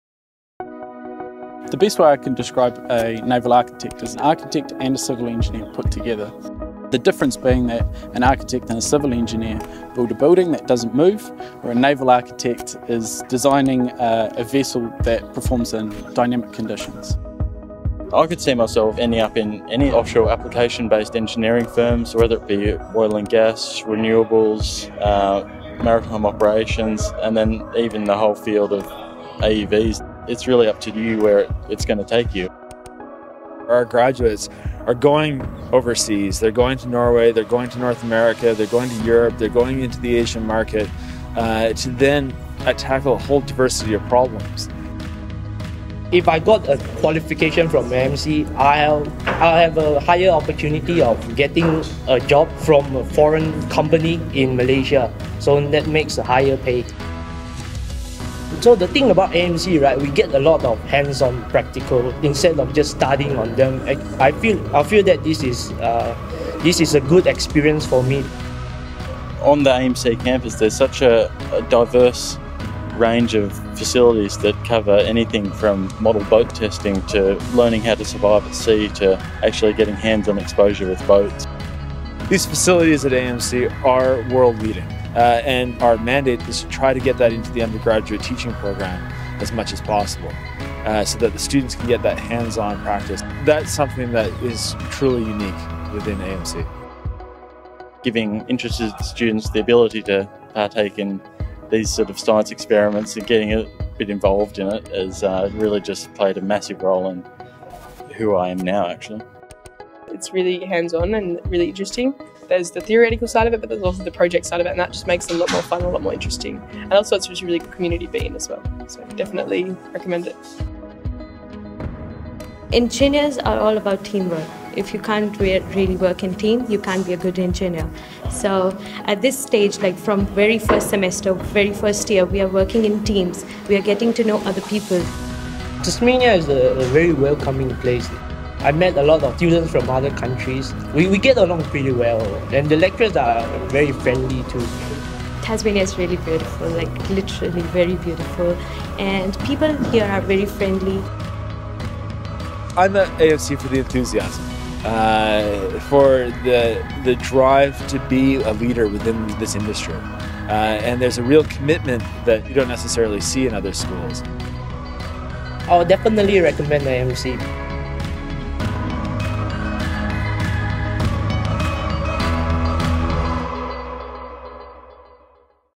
Maritime Engineering at AMC
The best way to describe a naval architect is an architect and a civil engineer put together.

AMC student Bryden Reay talks about his experiences studying with University of Tasmania.

"Tasmania is a very welcoming place. I met a lot of students from other countries."